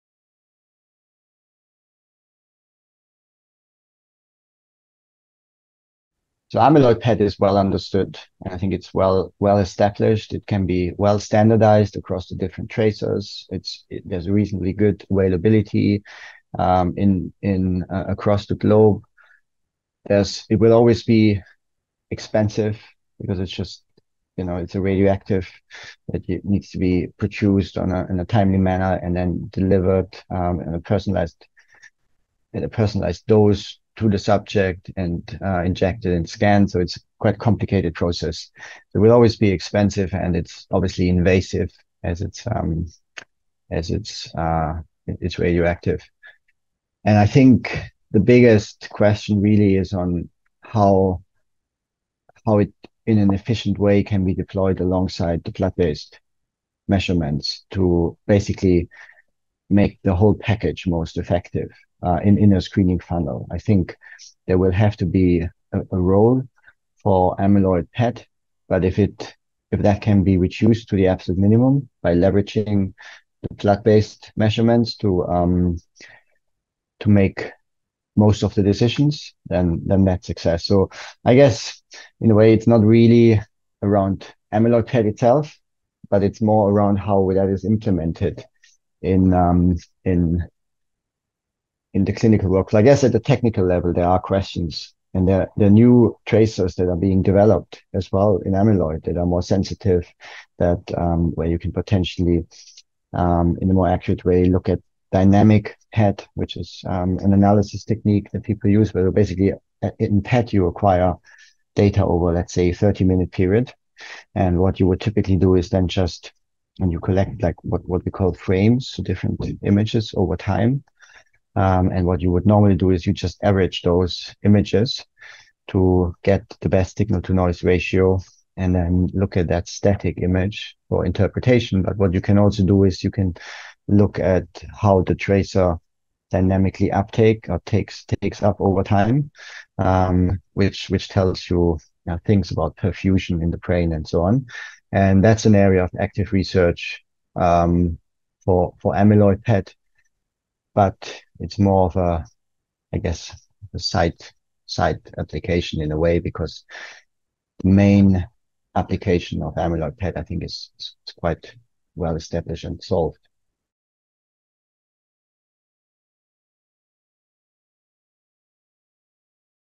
The future of Alzheimer’s disease diagnosis: blood biomarkers and amyloid-PET
Robin Wolz, PhD, IXICO, London, UK, shares his thoughts on the next steps for amyloid-PET research and implementation. Amyloid-PET has long been considered the gold standard approach for Alzheimer’s disease diagnosis and while it offers high accuracy, its use is limited by cost and availability. How best to utilize blood-based biomarkers alongside PET to minimize cost and invasiveness is a key area of investigation. The introduction of blood-based biomarkers has the potential to revolutionize the diagnostic and prognostic work-up of AD, enabling timely and accurate diagnosis whilst keeping PET requirement to a minimum. This interview took place at the CTAD 2023 conference in Boston, MA.